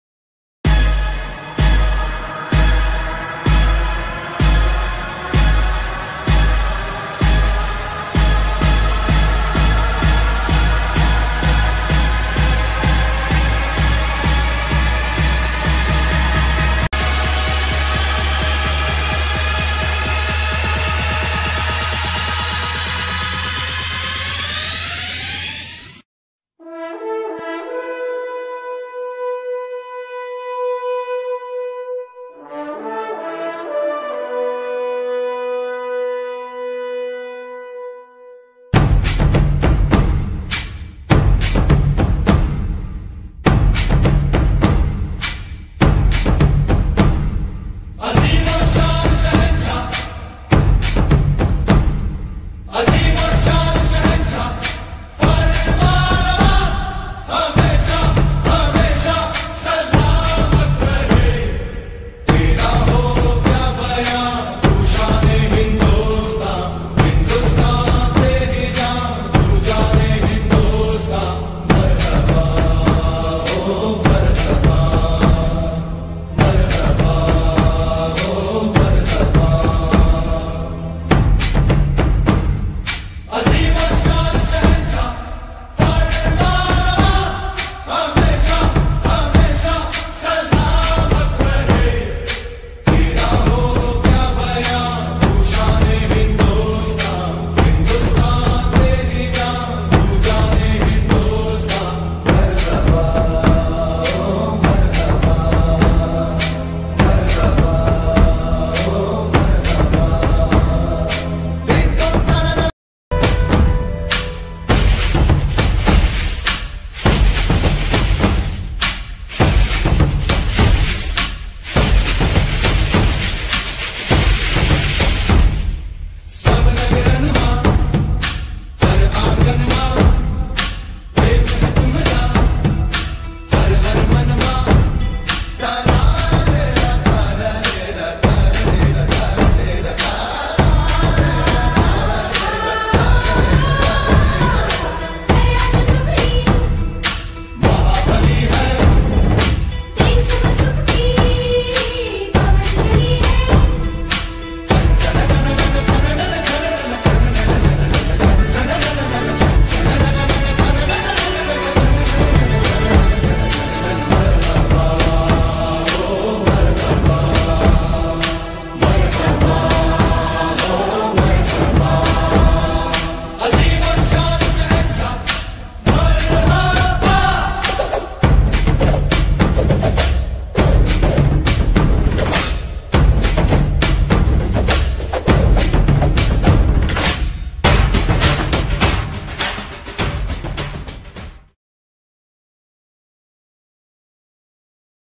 2022 Birthday Celebrations Of Syed Jahangir Hussain Sahab Senior Leader Qutubullapur Constituency
2023 Birthday Celebration Of Alhaj Syed Jahangir Hussain Sahab On (17th August 2023)